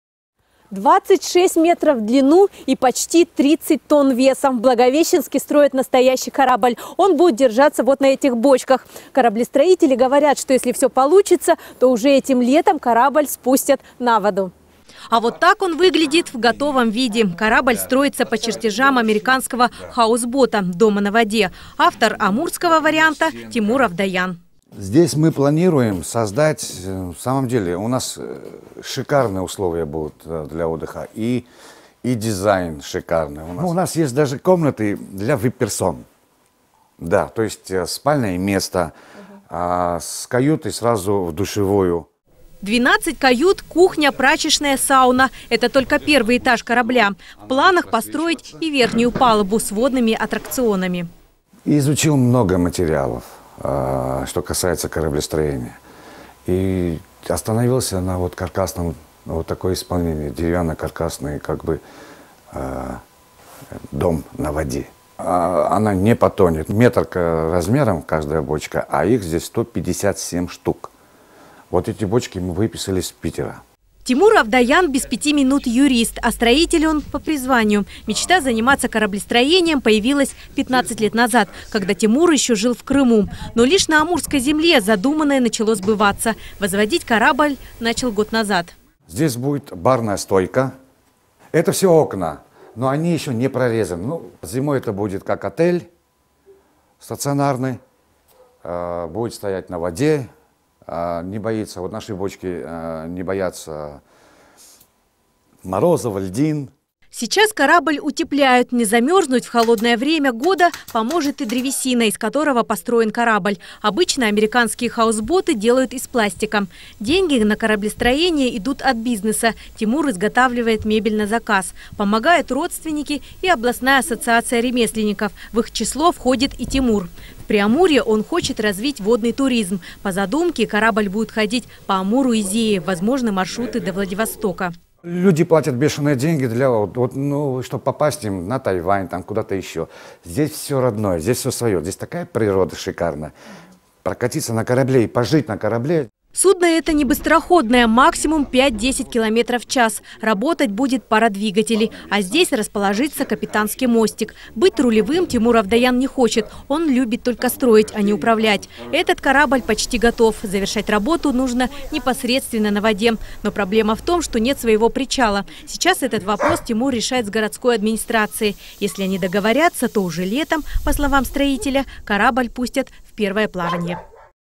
Благовещенец строит плавучий дом
В ассортименте кухня, сауна и водные аттракционы Ссылка на сайт / Видео предоставлено ТК "Альфа канал"